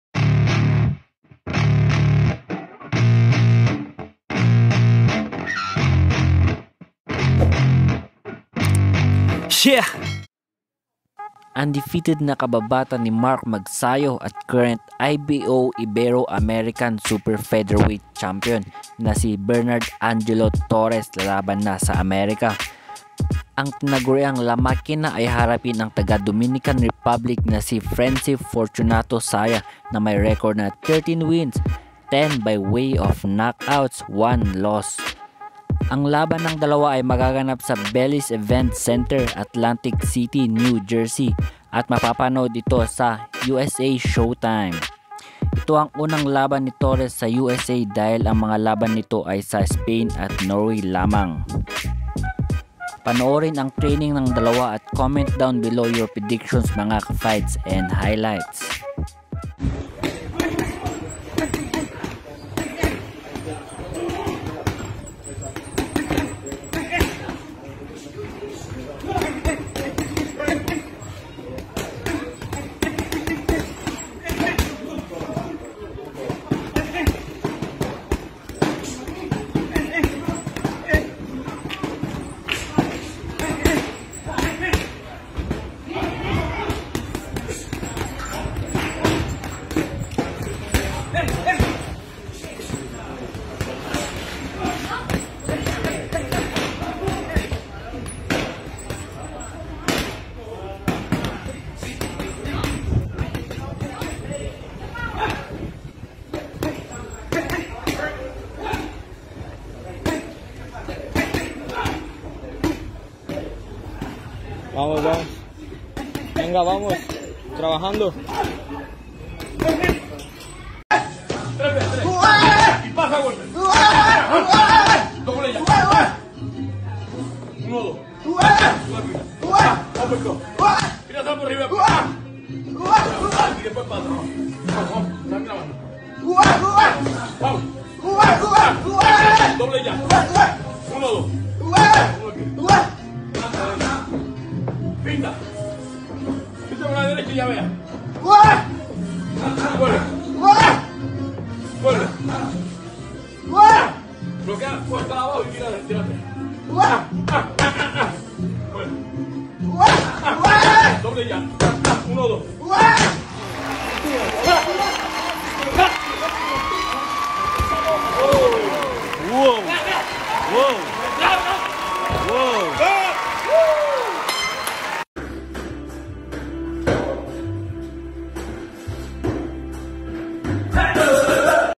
Undefeated Na Si Bernard Angelo Torres Lalaban Na Sa America Vs Frency Fortunato Saya
Bernard Torres Vs Frency Fortunato Saya